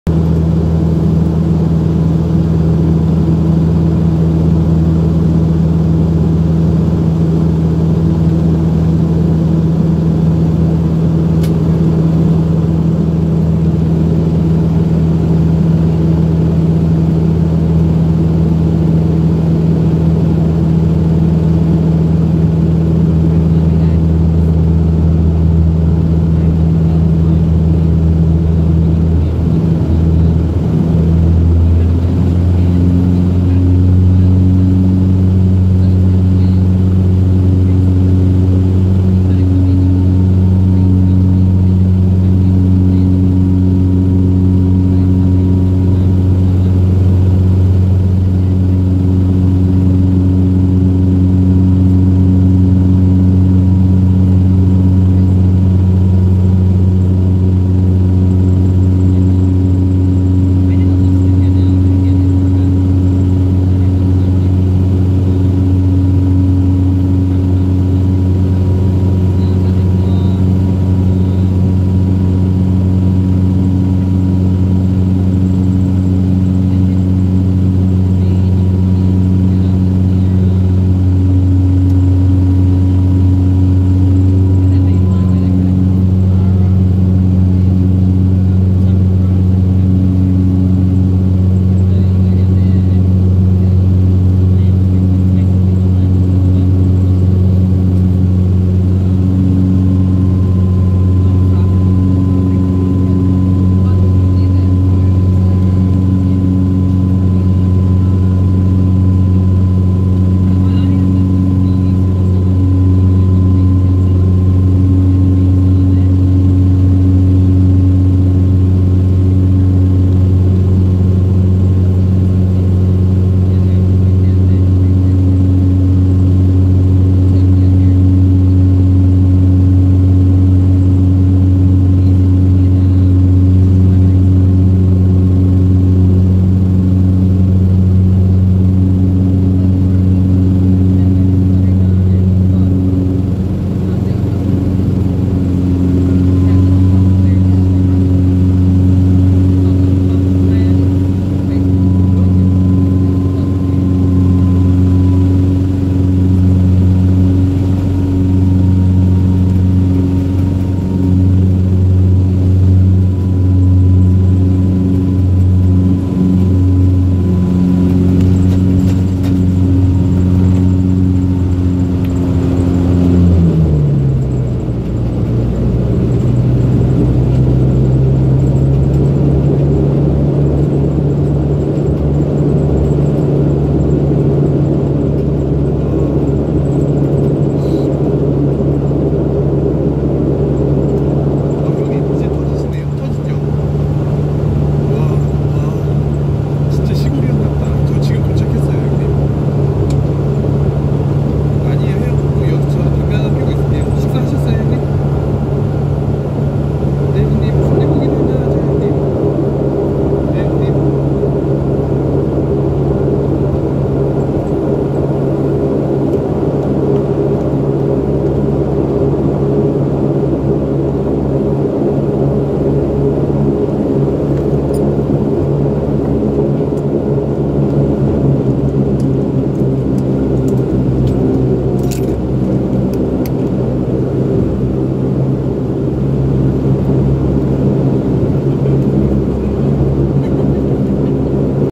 4K Rex landing Brisbane Saab 340 from Toowoomba Wellcamp
Date: 9th December 2018
Airline: REX
Flight No: ZL5632
Type: Saab 340
Aircraft: VH-ZLQ
Route: Toowoomba Wellcamp to Brisbane
Seat No: 3A
Flight time: 20mins
Landing runway: 16

Filmed with a Samsung S9 at 4K/UHD 3840x2160 @60fps